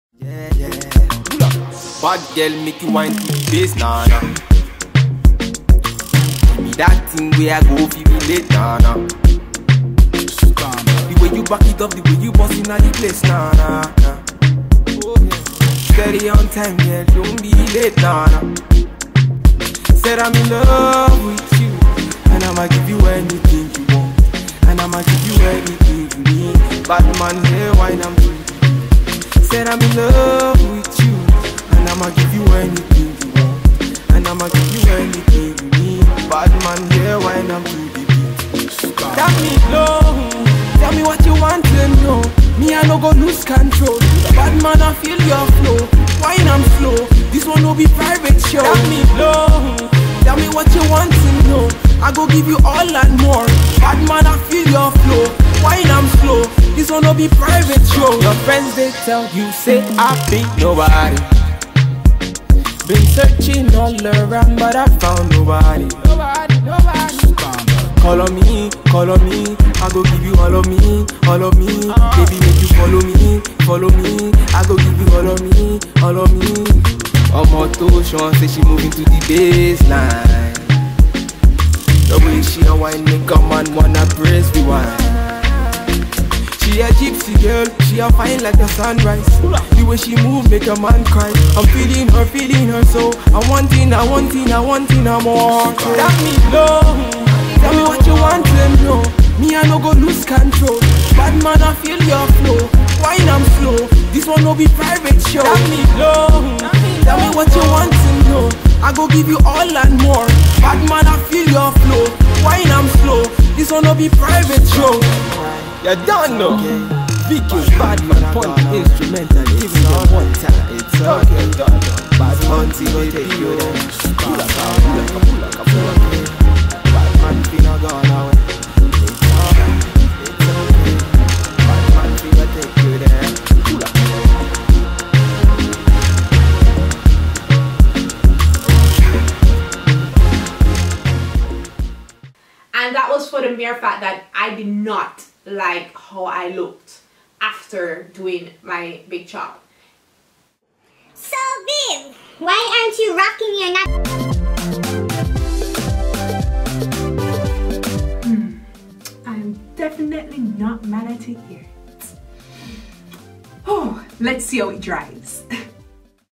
2 Years Natural LENGTH CHECK | Oct. 2017
Last relaxer - April 30, 2015
Big chop date - Oct. 23, 2015
Hair type - FRIZZY
My height - 5' 8.5"
Day job - Civil Engineer (bi-atch!!!)
Like my Facebook page: Hair Therapy for Jamaica

Please have title as: ATTN: YouTube Business